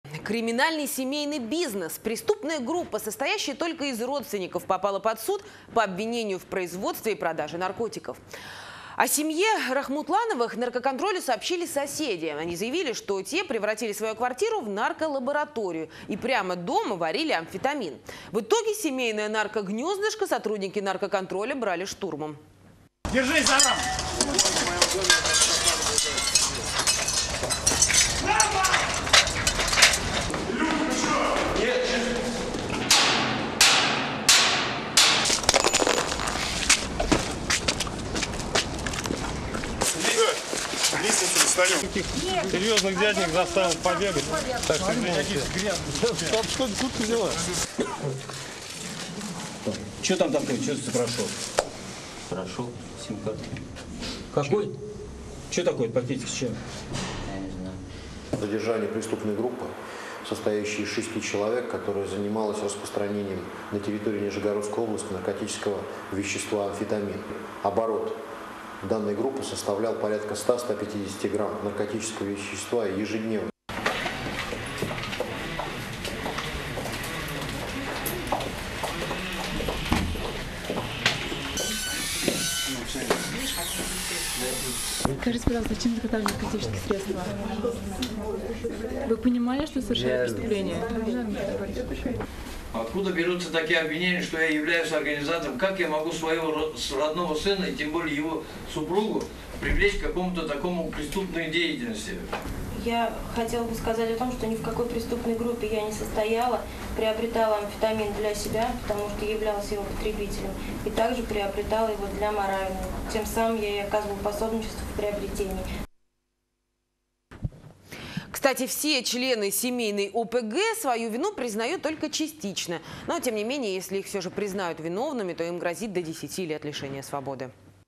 Криминальный семейный подряд - целая семья оказалась в суде по подозрению в производстве наркотиков
Криминальный семейный бизнес - преступная группа, состоящая только из родственников попала под суд по обвинению в производстве и продаже наркотиков. О семье Рахматуллаевых  наркоконтролю сообщили соседи - они заявили, что те превратили свою квартиру в нарколабораторию, и прямо дома варили амфетамин. В итоге, семейное нарко-гнездышко сотрудники наркоконтроля брали штурмом